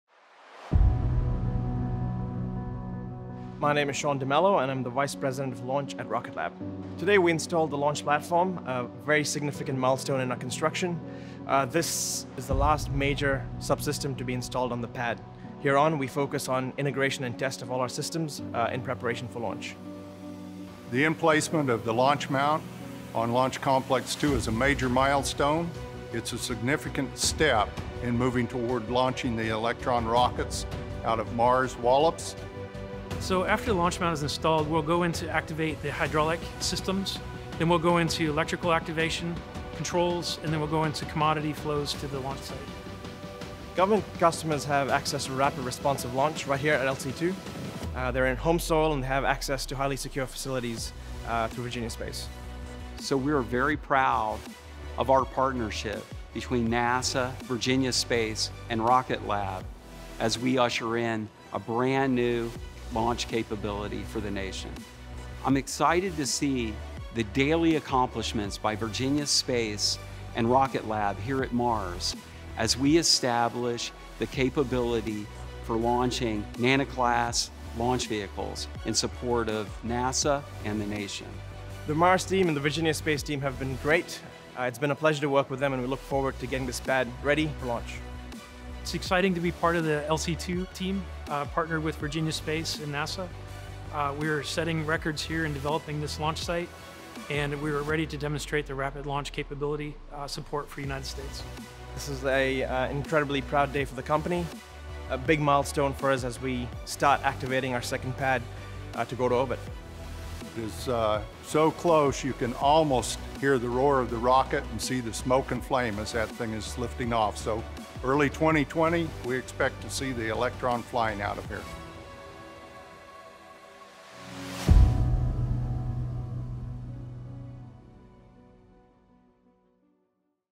Rocket Lab Launch Complex 2 - Launch Platform Installation
The recent installation of the launch platform at Rocket Lab’s second launch site, Launch Complex 2, marks one of the final steps in the construction of the new pad being built by the Rocket Lab and Virginia Space teams.